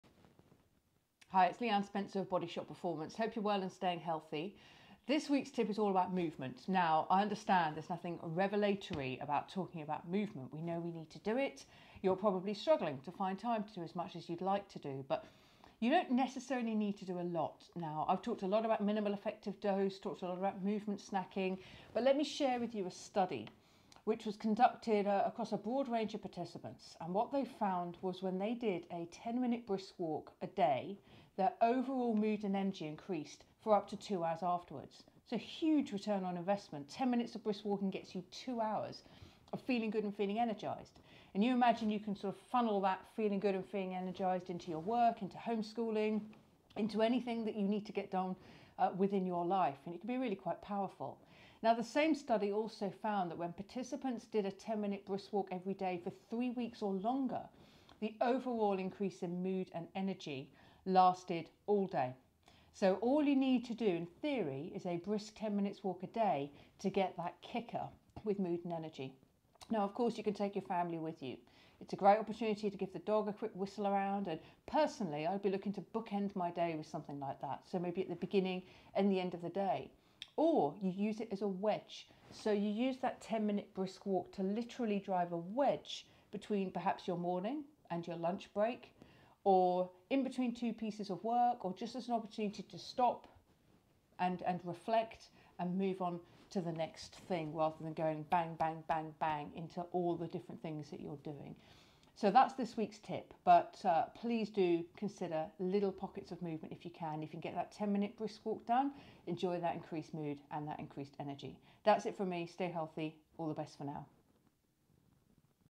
Thrive In Five Health Benefits of 10 Minutes of Movement
This Week’s Thrive In Five Is About The Health Benefits Of 10-Minutes Of Movement
So Thrive In Five tips are something I can impart in 5 minutes or less, that you can think about or implement in 5 minutes or less. This week’s tip is all about movement and specifically the health benefits of 10-minutes of movement.

Now I understand there’s nothing revolutionary about talking about movement for health and wellbeing. We know we need to do it. You’re probably struggling to find time to do as much exercise and movement as you’d like to do, but you don’t necessarily need to do a lot.

What Impact Can As Little As 10-Minutes Of Brisk Walking Have?
Now I’ve talked a lot about minimal effective dose talked a lot about movement snacking, but let me share with you a study, which was conducted across a broad range of participants. What the study found was when participants did a 10-minute brisk walk a day, their overall mood and energy increased for up to two hours afterwards.

So that’s a huge return on investment, just 10 minutes of brisk walking gets you two hours of feeling good and feeling energised. Imagine you can funnel that feeling good and feeling energised into your work and to homeschooling, into anything that you need to get done within your life.

It can be really quite powerful.

What Are The Longer-Term Health Benefits Of 10-Minutes Of Movement
Now the same study also found that when participants did a 10 minute brisk walk every day for three weeks or longer, the overall increase in mood and energy lasted all day.

So all you need to do in theory is a brisk 10 minutes of movement, perhaps a walk or something else, a day to get that upswing in mood and energy.

Now, of course, you can take your family with you. It’s a great opportunity to give the dog whistle around.

Personally, I’d be looking to bookend my day with 10-minutes of movement. So maybe at the beginning and the end of the day, or you use it as a wedge. So you use that 10-minutes of movement to literally drive a wedge between perhaps your morning and your lunch break or in between two pieces of work. Or just as an opportunity to stop and reflect and move on to the next thing. Rather than going bang, bang, bang, bang into all the different things that you’re doing.

So that’s this week’s tip, but please do consider little pockets of movement. If you can get that 10-minutes of movement done, and enjoy that increased mood and that increased energy. That’s it from me, stay healthy all the best for now.